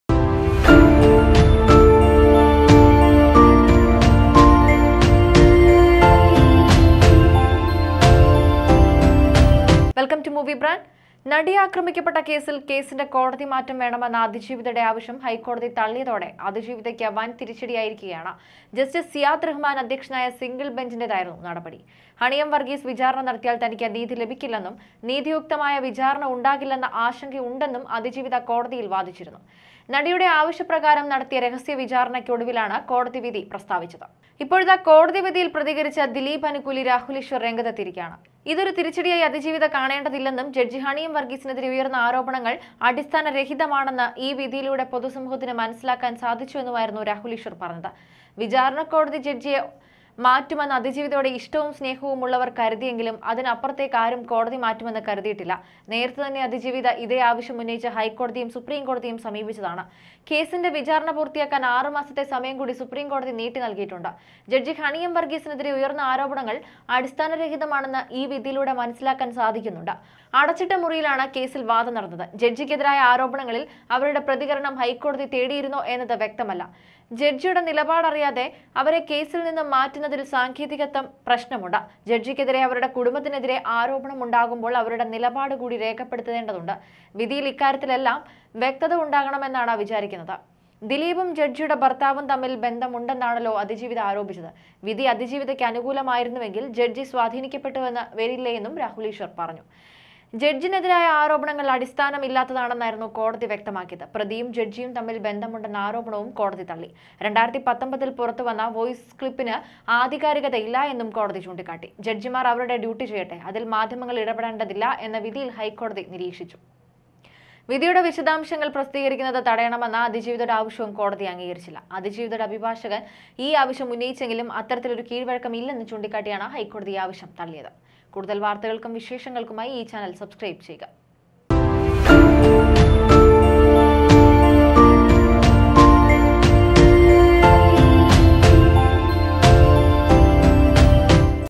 Dileep | Rahul നെഞ്ചിൽ തറച്ച് ആ വാക്കുകൾ
ജഡ്ജിക്കെതിരെ അവരുടെ കുടുംബത്തിനെതിരെ ആരോപണം ഉണ്ടാകുമ്പോൾ അവരുടെ നിലപാട് കൂടി രേഖപ്പെടുത്തേണ്ടതുണ്ട്... വിധിയിൽ ഇക്കാര്യത്തിലെല്ലാം വ്യക്തത ഉണ്ടാകുമെന്നാണ് വിചാരിക്കുന്നത്.. ദിലീപും ജഡ്ജിയുടെ ഭർത്താവും തമ്മിൽ ബന്ധം ഉണ്ടെന്നാണല്ലോ അതിജീവിത ആരോപിച്ചത്.. വിധി അതിജീവിതയ്ക്ക് അനുകൂലമായിരുന്നുവെങ്കിൽ ജഡ്ജി സ്വാധീനിക്കപ്പെട്ടുവെന്ന് വരില്ലേ...